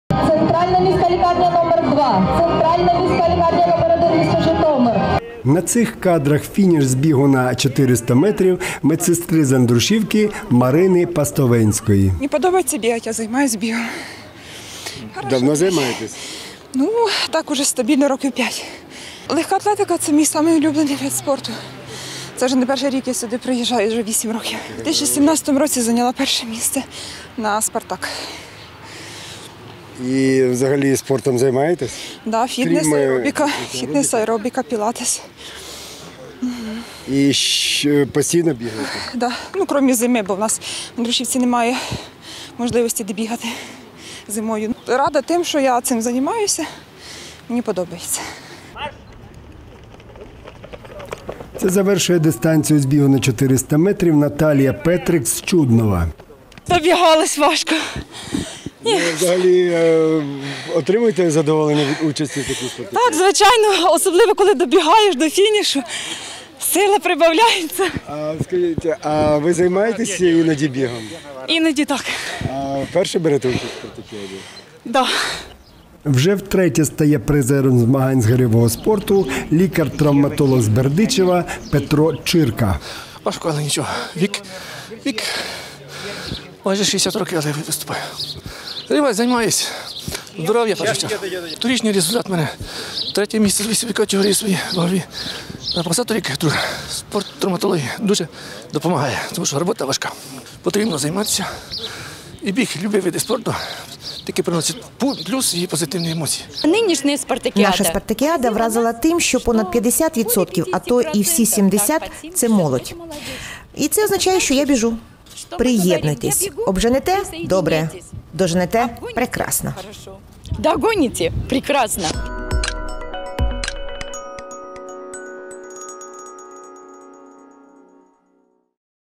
Відбулися XXVI спортивні ігри серед медпрацівників області_Канал UA: ЖИТОМИР 29.05.19
XXVI спортивні ігри серед медичних працівників області відбулись на  спортивній базі Житомирського агротехколеджу.

32 колективи медичних працівників з різних районів області змагались з армреслінгу, в гирьовому спорті, з шахів та шашок, настільного тенісу та легкої атлетики.